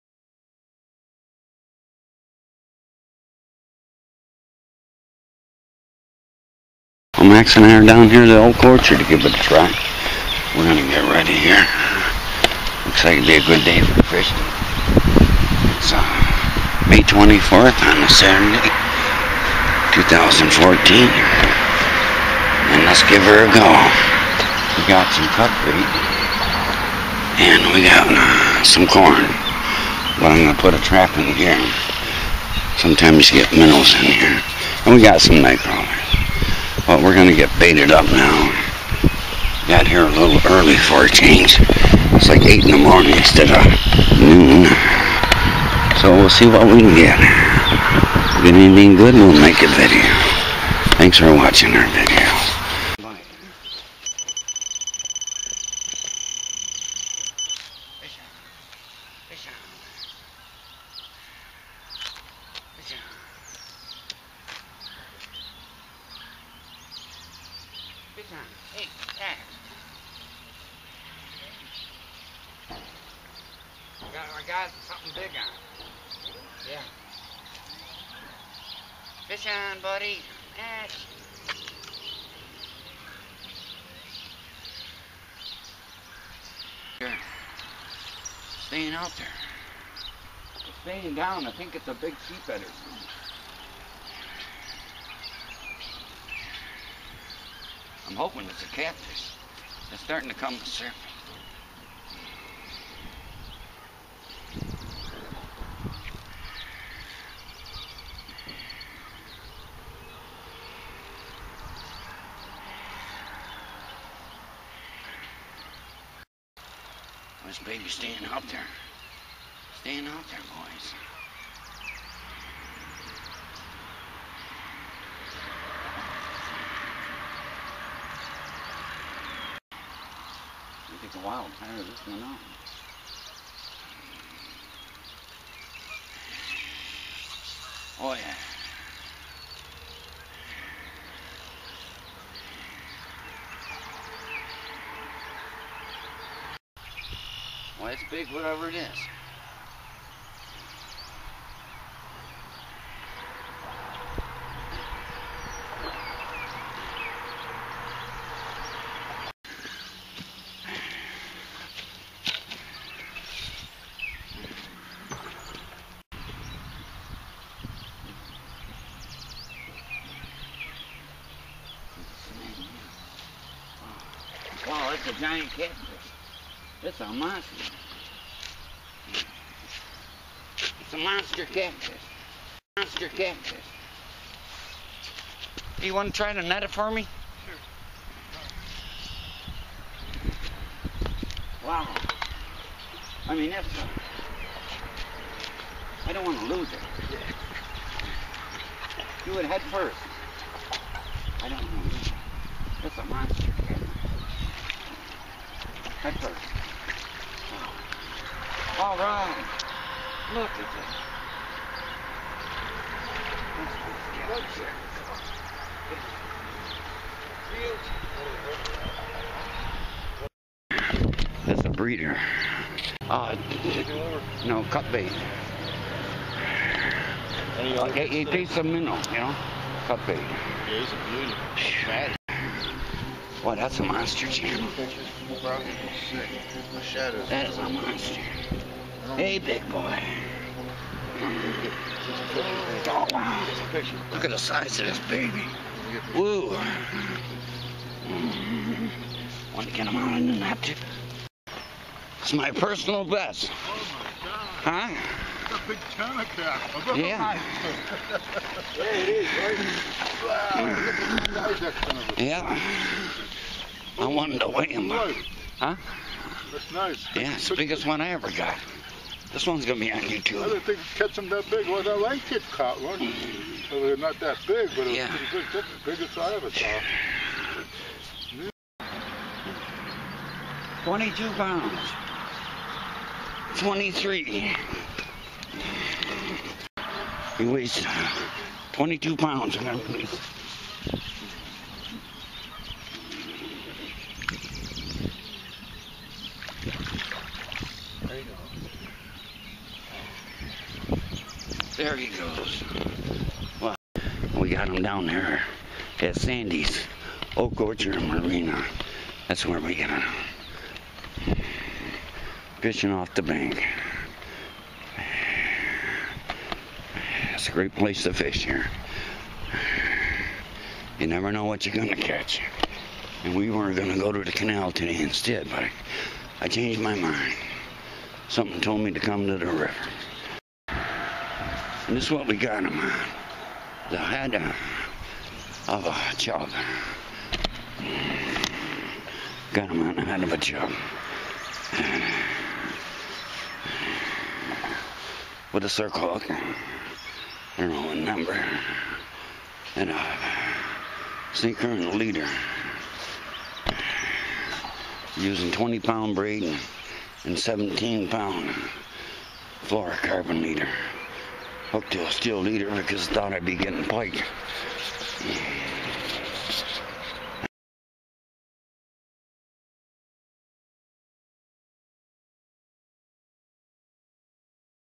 Bank Fishing Monster 22 Lbs Channel Catfish
Fishing At Sandys Marina On The Oak Orchard River Got Monster 22lbs Male Channel Catfish. Big big thanks to Keith Umile from Pa for Netin The Catfish for me. Don't let the Game Vultures get you!